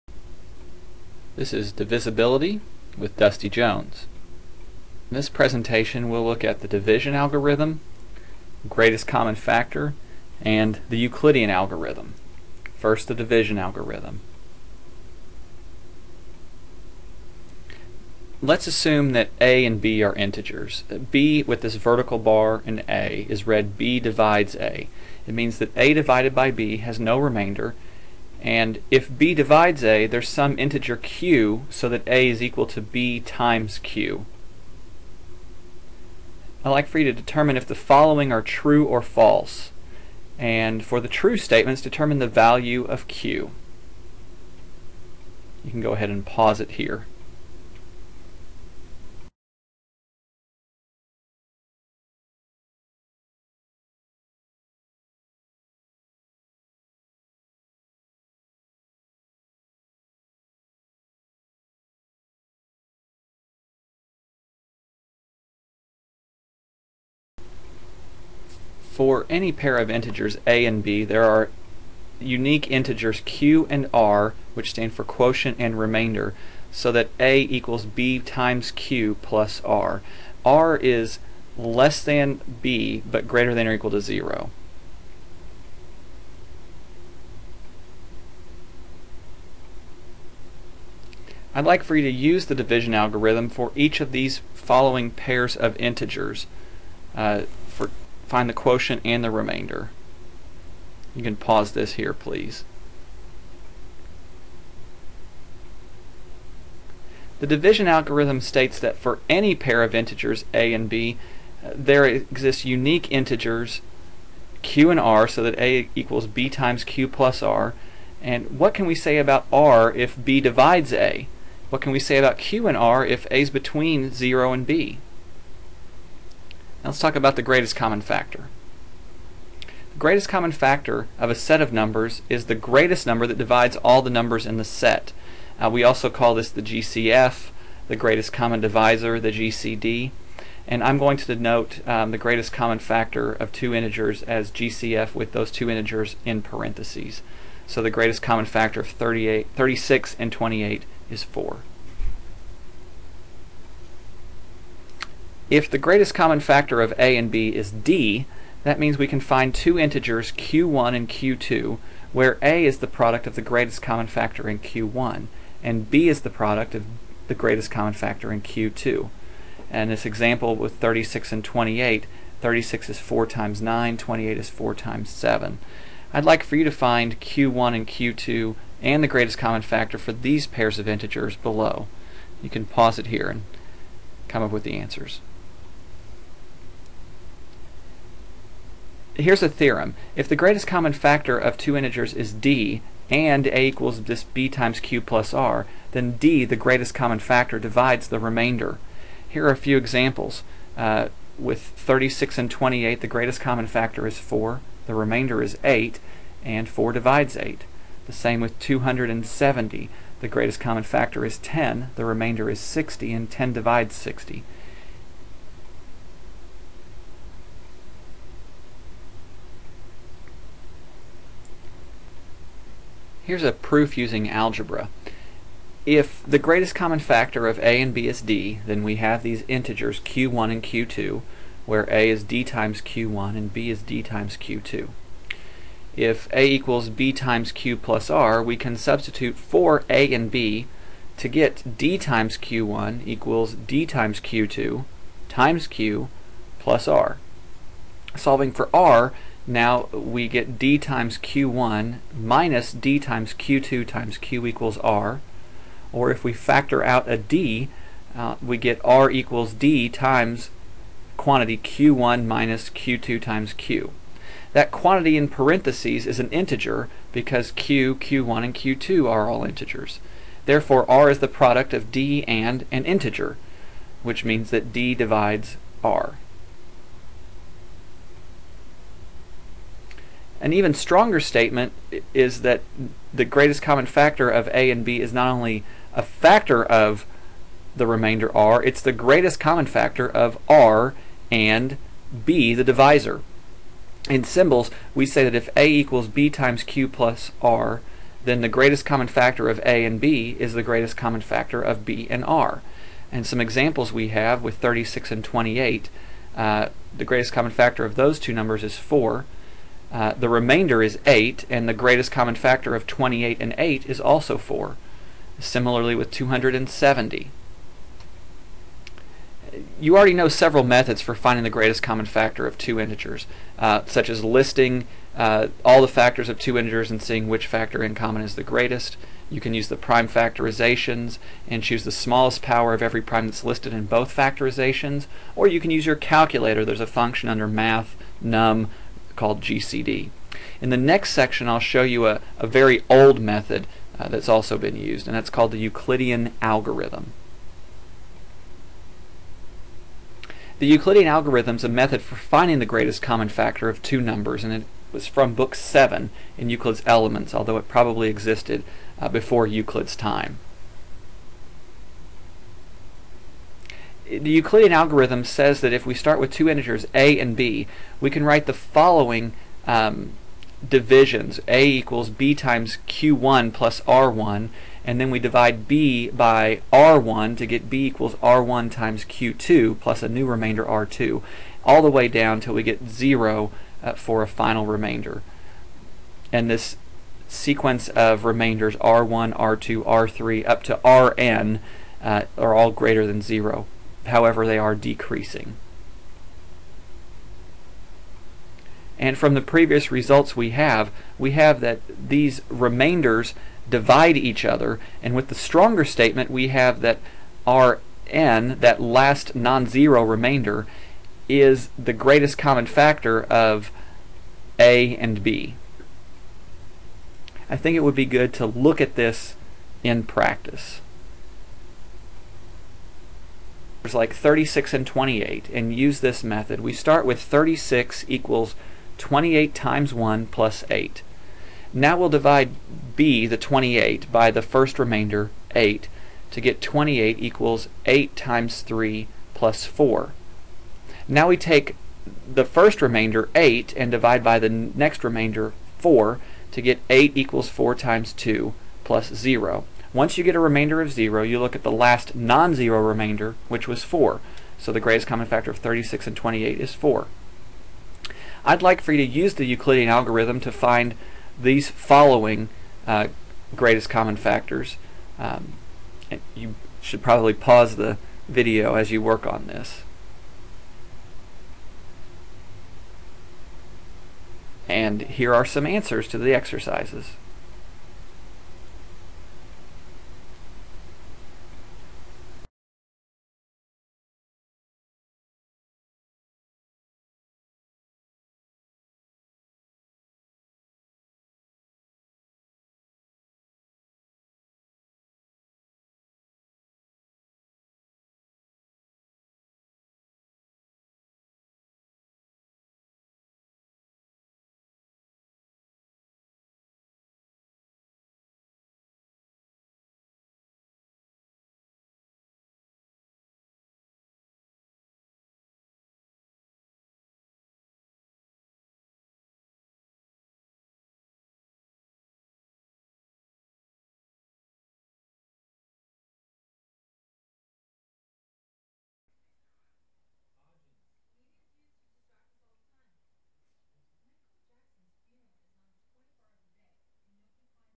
Divisibility
Divisiblity, the division algorithm, greatest common factor, and the Euclidean algorithm